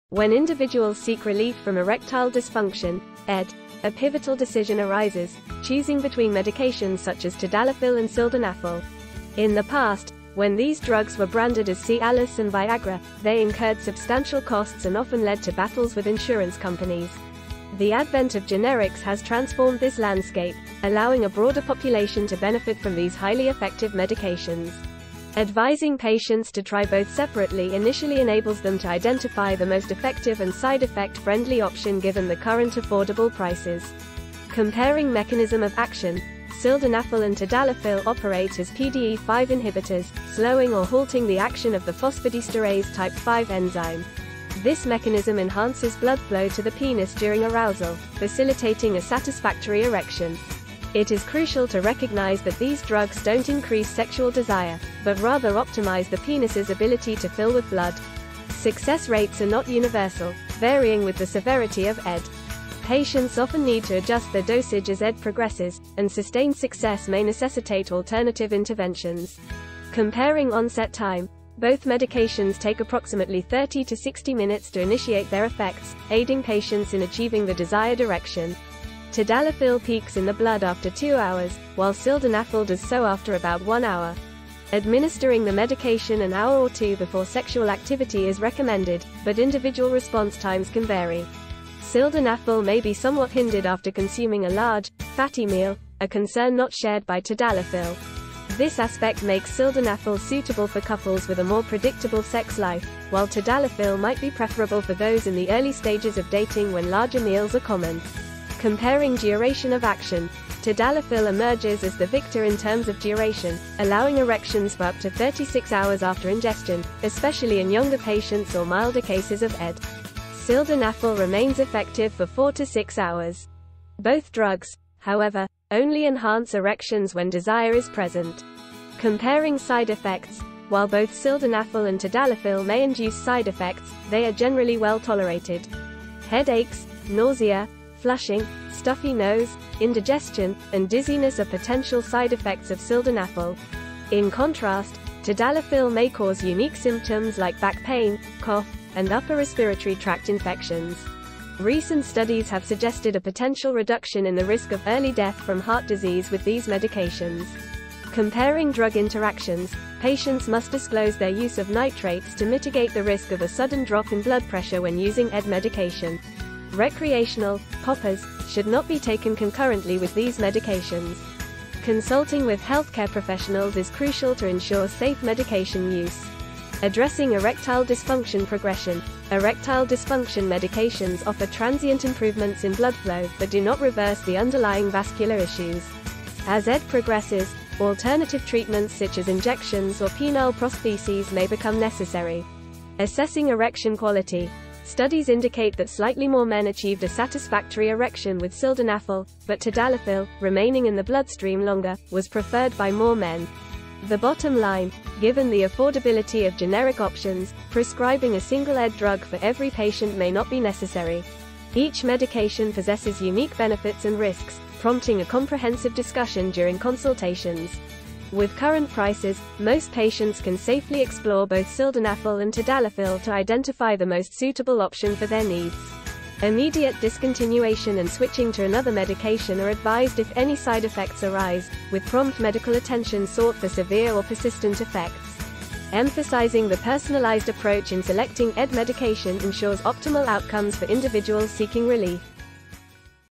Everything you need to know about Sildenafil vs Tadalafil for erectile dysfunction
🌐 Unlocking the Secrets to Erectile Dysfunction Medications: Sildenafil vs. Tadalafil

🔍 Explore the world of erectile dysfunction (ED) medications with our in-depth analysis of two popular choices—sildenafil and tadalafil. In this comprehensive video, we unravel the nuances of these medications, providing valuable insights to empower individuals seeking relief from ED.

👨‍⚕️ Join us as we delve into the historical context, from the days of branded Cialis and Viagra to the present, where affordable generic options have transformed accessibility for a wider audience. Learn about the strategic recommendation of trying both medications separately, allowing users to discern the most effective and side-effect-friendly solution, especially with today's affordable pricing.